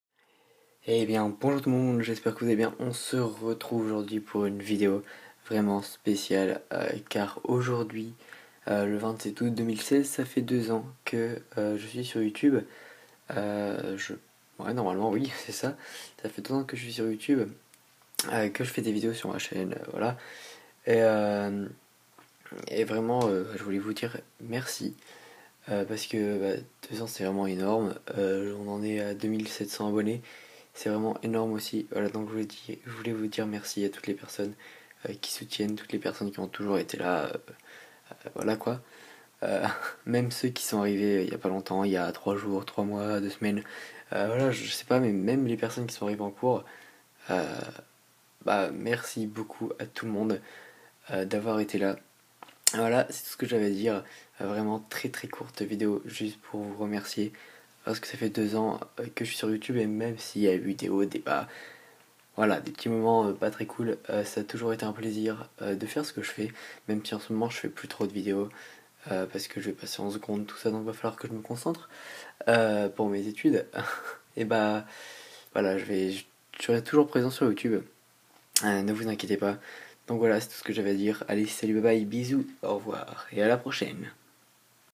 2 ANS DE LA CHAINE !!!
Hey Bonjour tout le monde j'espère que vous allez bien car moi ça va super et aujourd'hui on se retrouve pour une vidéo spéciale car aujourd'hui ça fait 2 ans que je suis sur YouTube !

Petites Infos
Mon twitter: @BaptsYTB
Mon Skype: Bapt's YTB

Merci a toi d'avoir regardé ;D